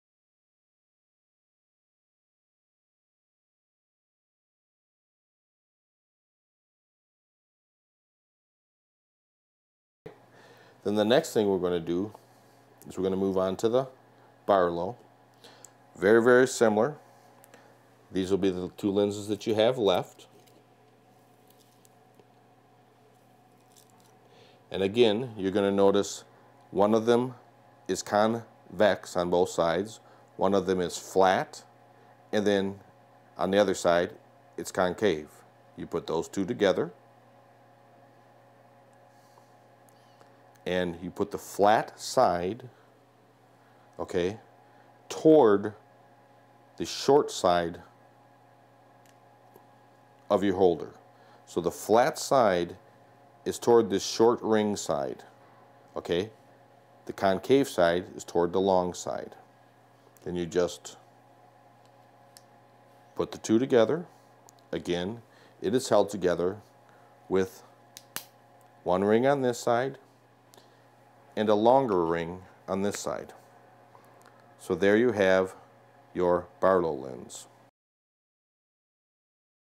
SkyViewer Assembly Part 3 - Barlow
Part three of the SkyViewer telescope instructions covers the assembly of the Barlow.